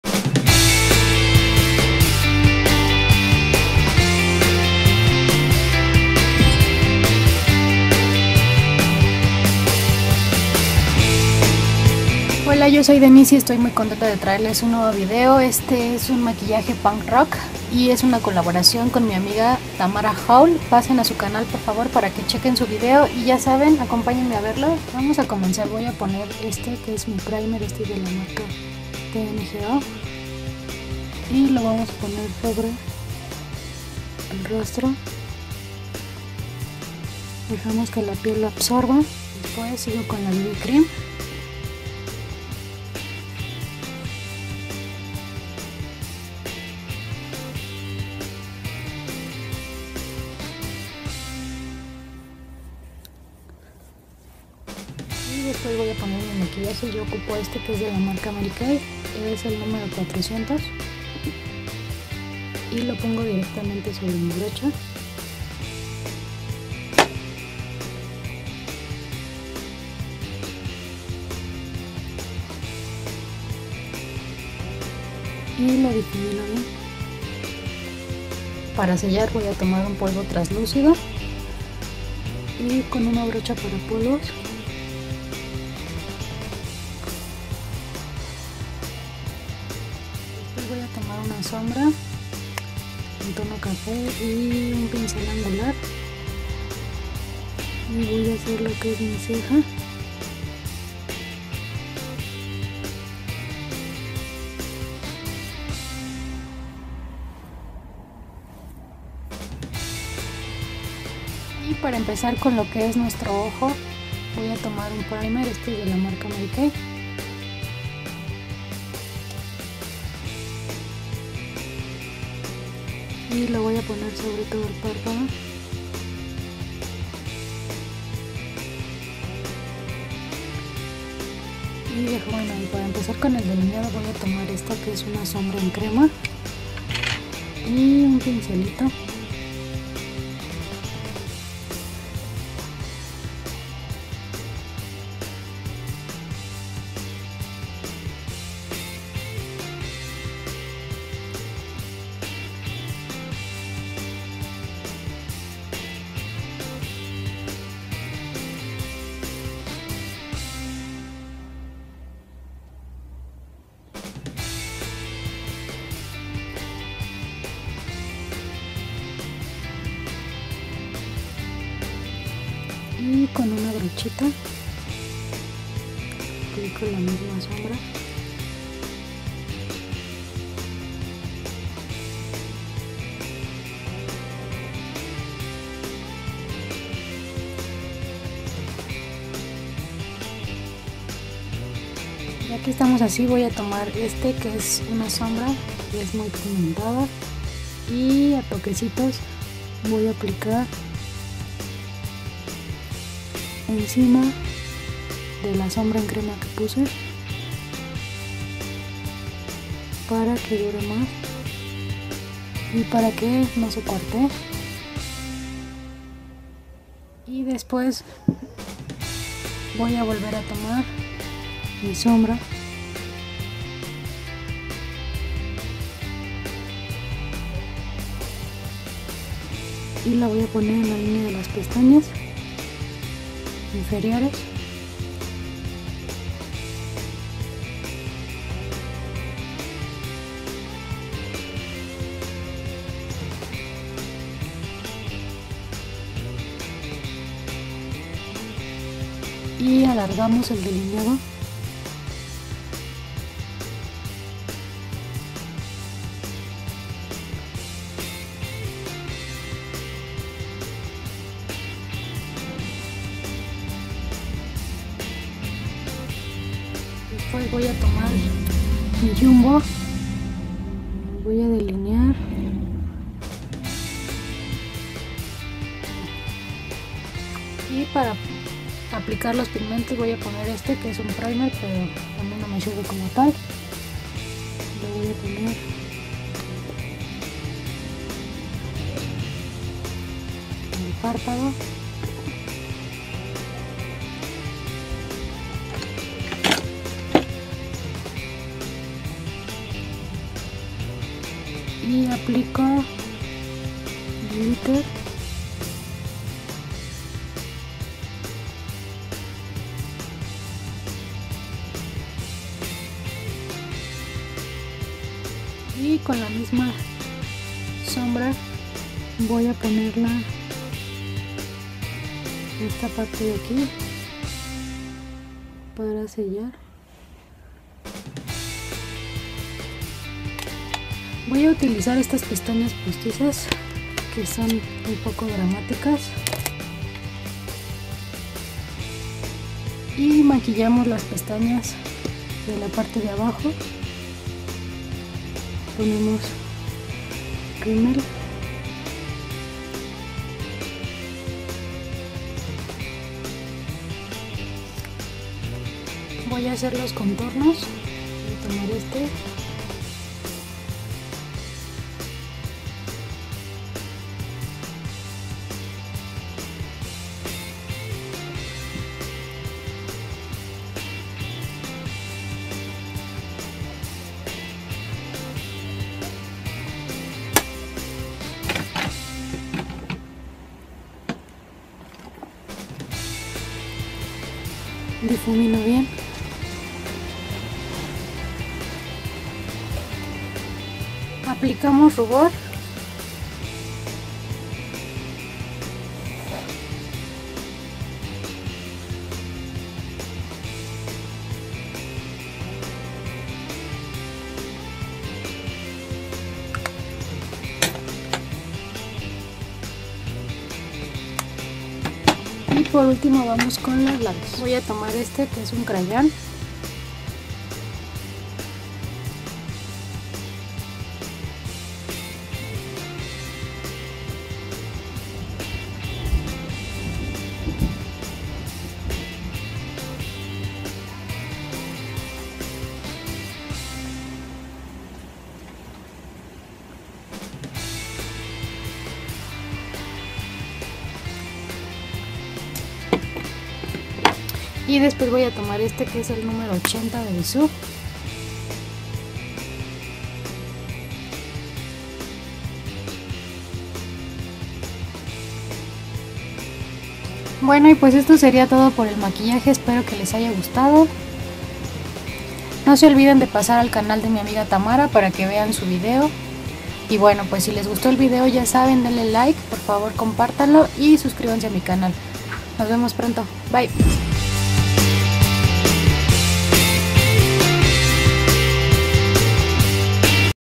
Makeup punk rock / maquillaje estiló rockero en colaboración con Támara Houle
Hola hoy les traigo este maquillaje en colaboración con mi amiga támara houle pasen a ver su video si te gusto el video dale like compártelo y suscribete a mi canal gracias !!!